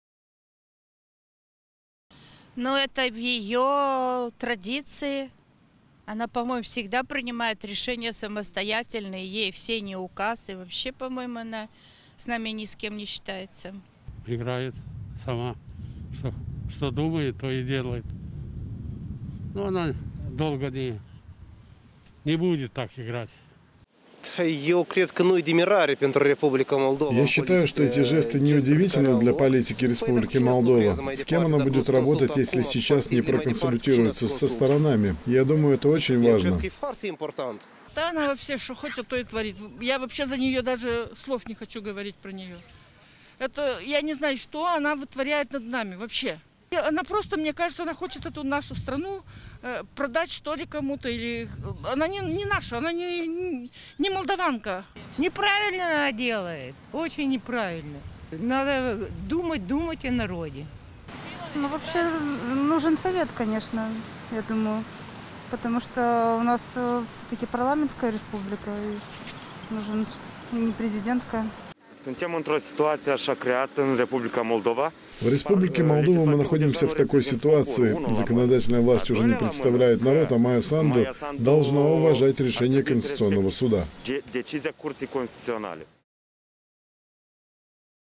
Vox Populi
Решение президента Майи Санду выдвинуть на пост премьер-министра Игоря Гросу, не посоветовавшись с праламентом, озадачило и граждан. Они говорят, что не могут объяснить жест президента и считают, что в кризисных ситуациях необходимо активизировать усилия, чтобы вывести страну из хаоса.

«Это в её традиции. Она, по-моему, всегда принимает решение самостоятельно, ей все не указ. И вообще, она, по-моему, ни с кем не считается».

 «Она играет сама. Что думает, то и делает. Но она долго не будет так играть».

«Я считаю, что эти жесты не удивительны для политики Республики Молдова. С кем она будет работать, если сейчас не проконсультируется со сторонами. Я думаю, это очень важно».

 «Она вообще, что хочет, то и творит, вообще не хочется о ней говорить. Что она вытворяет?  Она просто, мне кажется, хочет продать нашу страну кому-то. Она не наша, она не молдаванка».

«Не правильно она делает, очень не правильно. Надо думать о народе».

 «Ну вообще нужен совет, конечно, я думаю. У нас, всё-таки, парламентская республика, а не президентская».

 «В Республике Молдова мы находимся в такой ситуации –  законодательная власть уже не представляет народ, и Майя Санду должна уважать решение Конституционного суда».